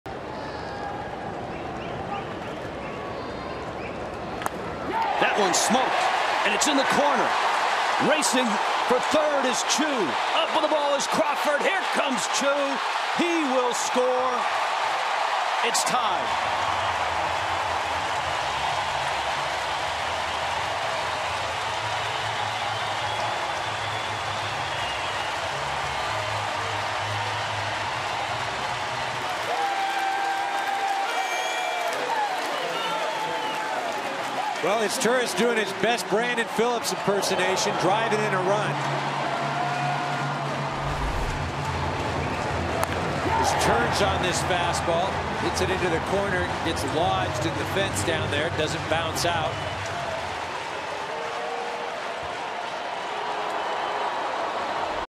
LAD@CIN: Izturis doubles to left to score Choo
9/7/13: Cesar Izturis rips a double into left field, scoring Shin-Soo Choo to tie the game at 3 in the bottom of the seventh